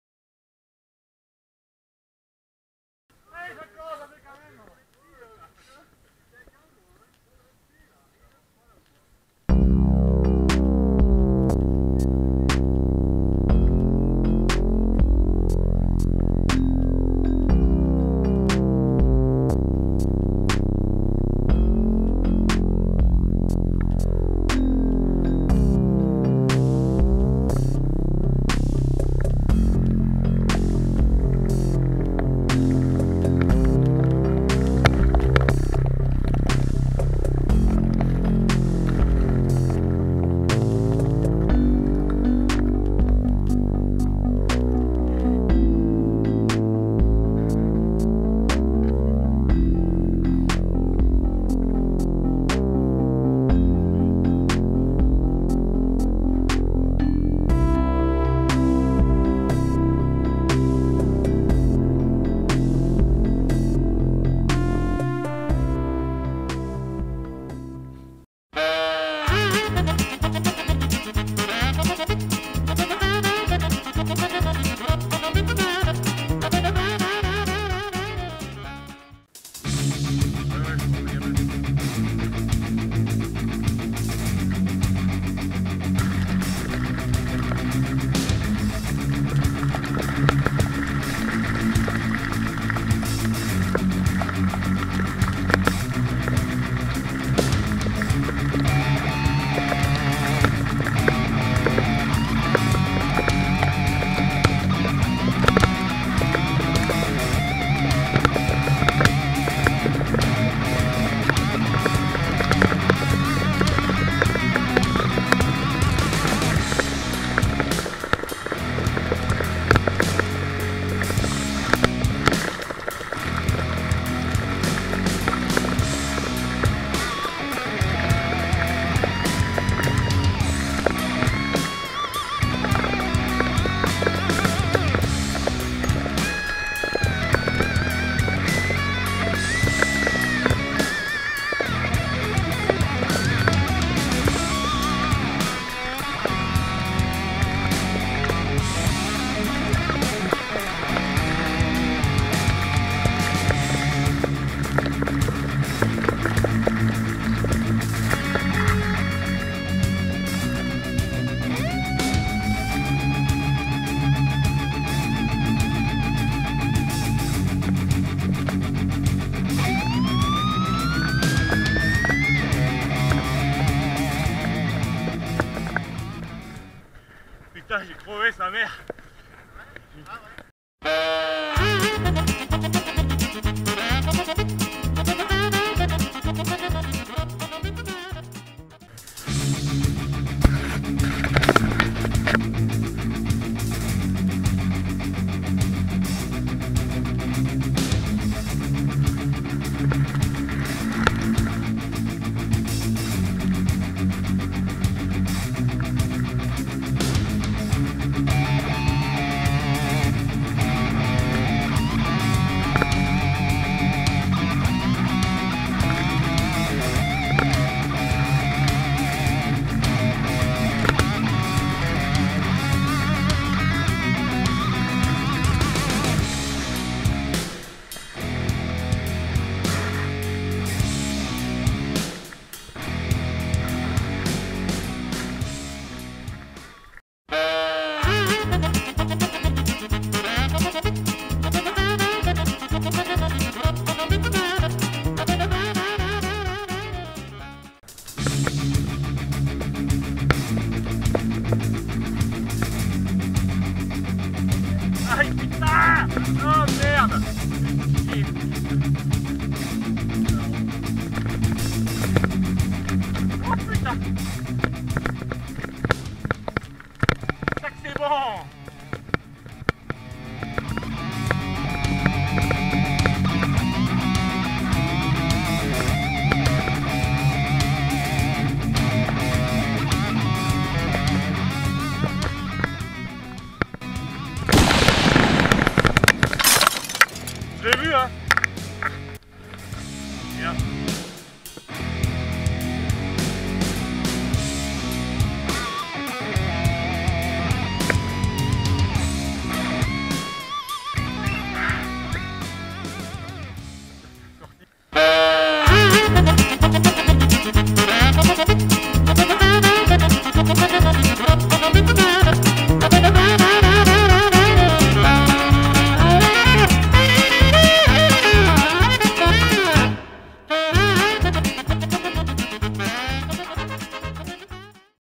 VTT - Tour de Saint Martin de la Cluze 21/06/2015
Tour de Saint Martin entre potes. Sortie chat noir en crevaisons, mais bien sympa. Spéciale dédicace à Matthieu, remets toi vite.